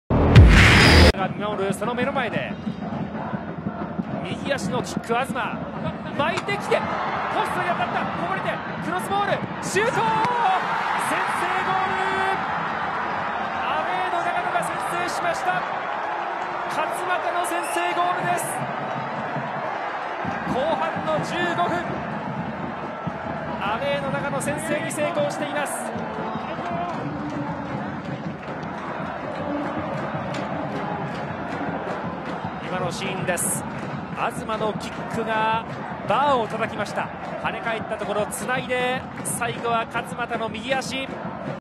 【公式】ゴール動画：勝又 慶典（長野）60分 ＳＣ相模原vsＡＣ長野パルセイロ 明治安田生命Ｊ３リーグ 第1節 2017/3/12
ポストを叩いたフリーキックのこぼれ球を長野が拾い、勝又 慶典（長野）が豪快なシュートをたたき込んで先制に成功！
2017年3月12日（日）に行われた明治安田生命Ｊ３リーグ 第1節 相模原vs長野のゴール動画です。